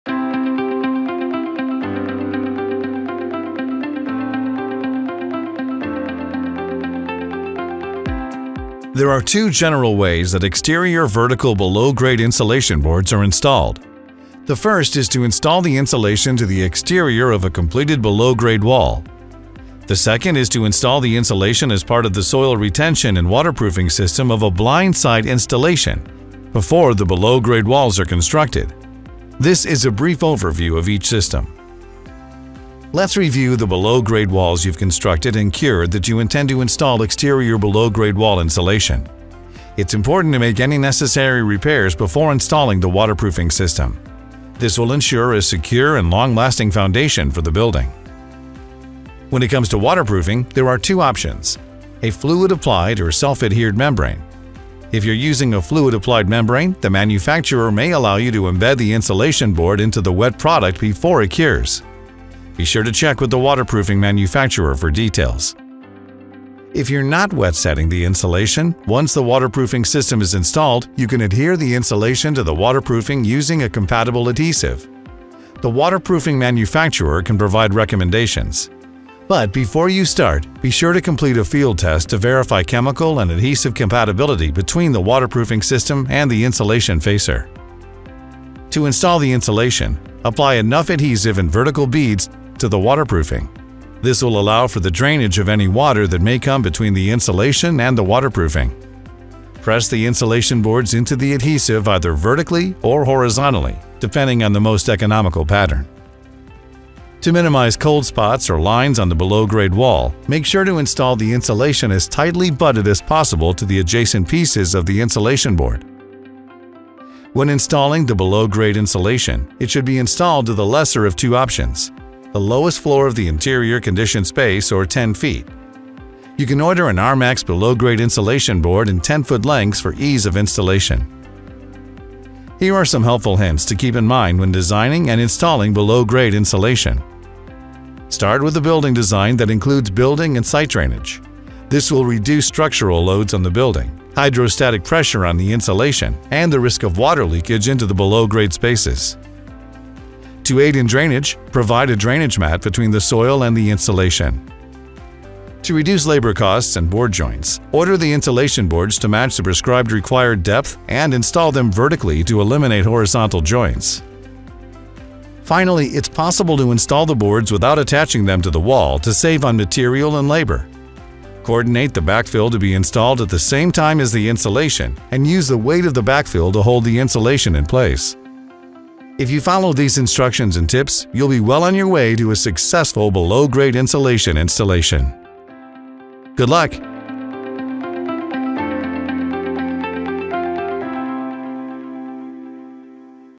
How to Install Rmax Below Grade Insulation | Standard Installation
Discover the step-by-step process for installing Rmax Below Grade insulation using the standard construction method. This video provides a comprehensive guide to achieving effective waterproofing and insulation for foundation construction in tight-quarter sites.

In This Video:

- Standard Technique: Detailed instructions for the standard construction method.
- Insulation Installation: Properly install below-grade insulation boards with adhesive or mechanical fasteners.
- Waterproofing System: Steps to install and integrate a waterproofing system with insulation.
- Tips & Best Practices: How to reduce cold spots, manage structural loads, and ensure effective drainage.

Key Features:

- Rmax Below Grade Insulation: Benefits and installation techniques for below-grade applications.
- Waterproofing Solutions: Combining insulation with waterproofing for optimal performance.
- Efficient Installation: Strategies to minimize labor costs and improve installation efficiency.